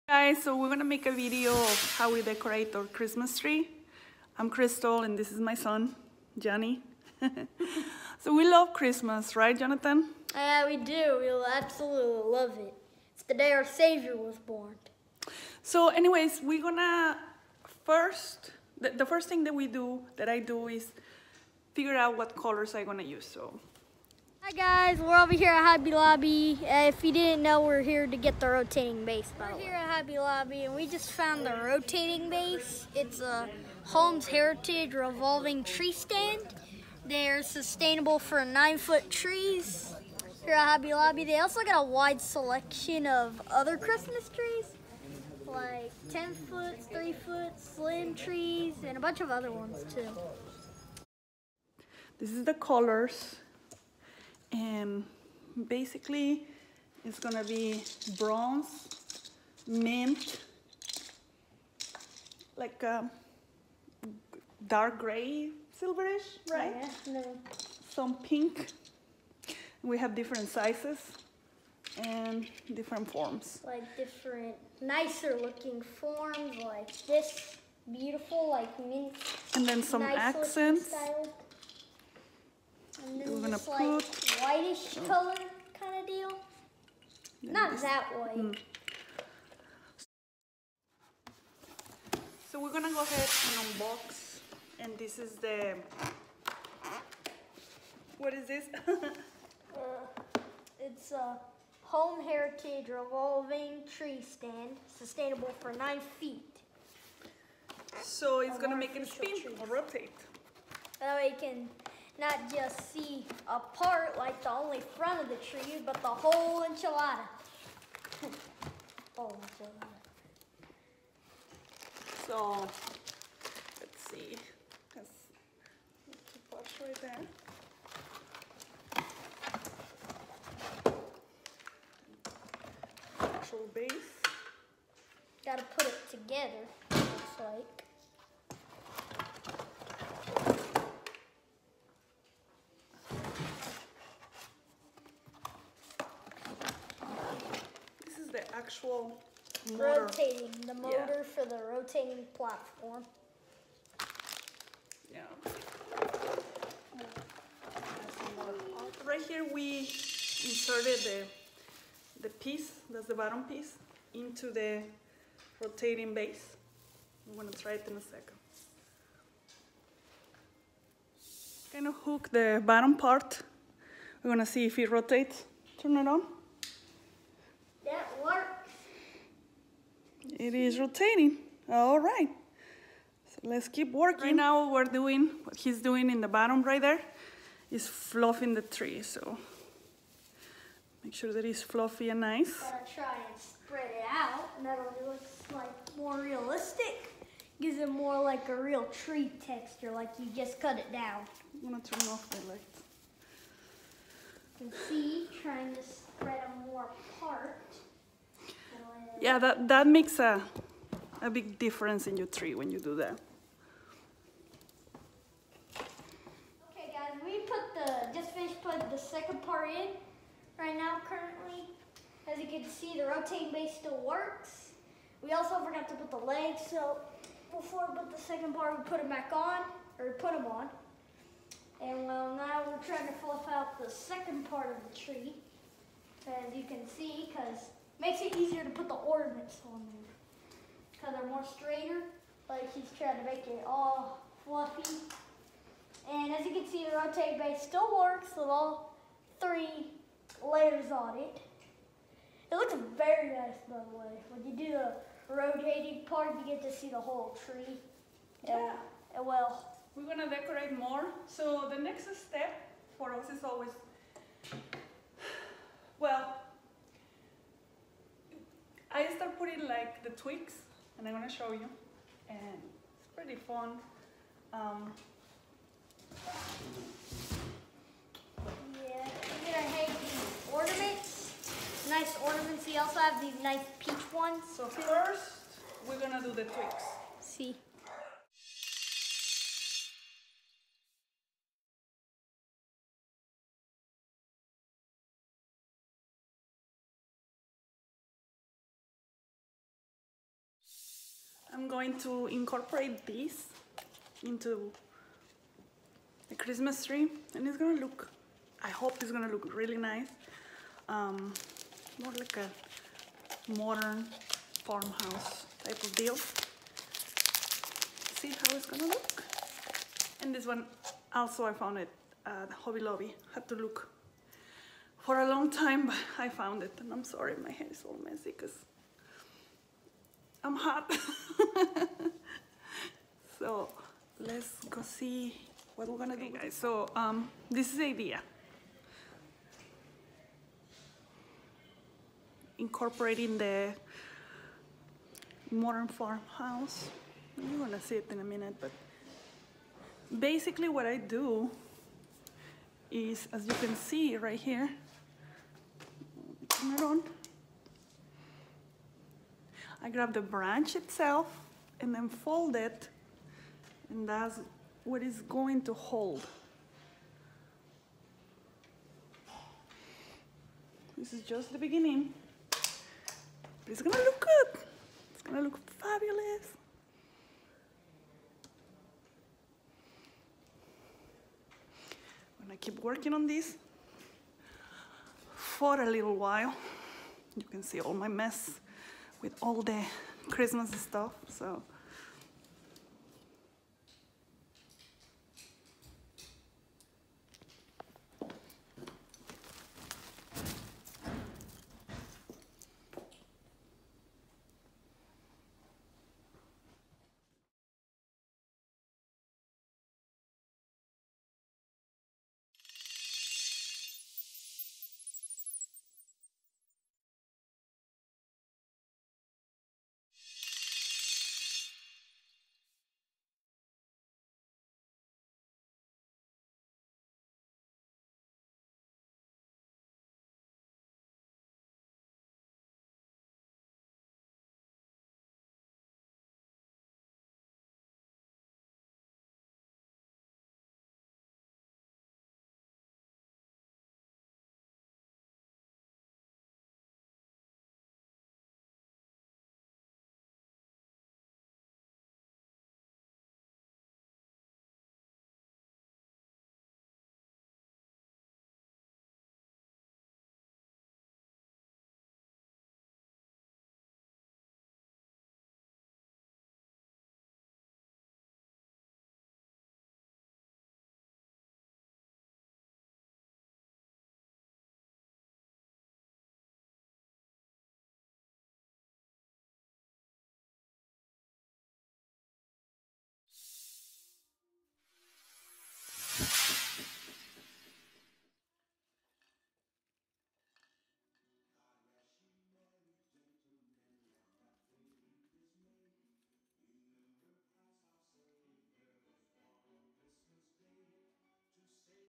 Christmas tree rotating base, modern farmhouse decor and French Bulldogs 2019
French bulldogs and modern Farmhouse Christmas decorations